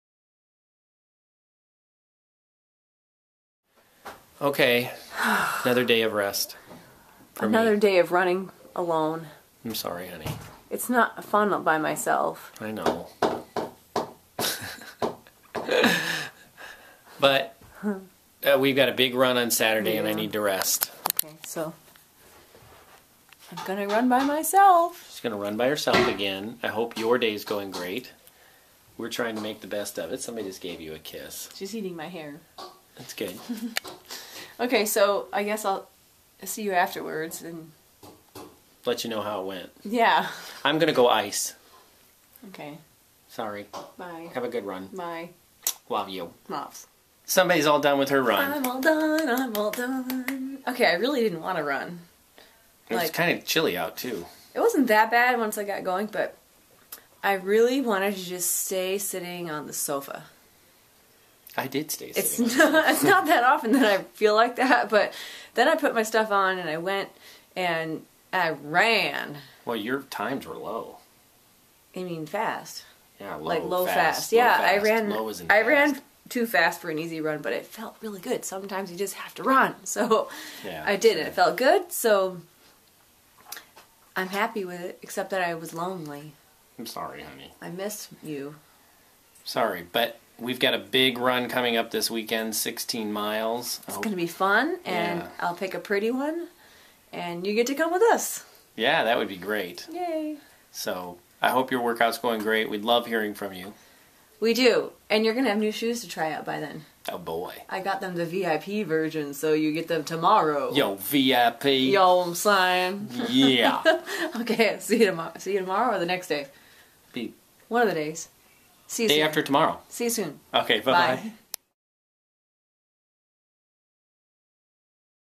TransRockies Run Training Day 74
Another solo run, this just won't do! I'm happy David is taking time off to heal, but I sure miss my running partner.